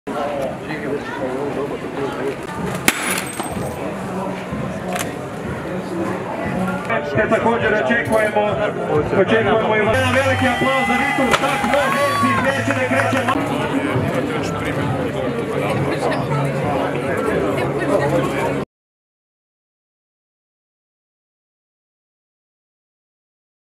Predsjednik Zoran Milanović prošetao Špancirfestom
Varaždin, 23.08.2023 - Predsjednik Zoran Milanović prošetao Špancirfestom. Video: Željko Hajdinjak / CROPIX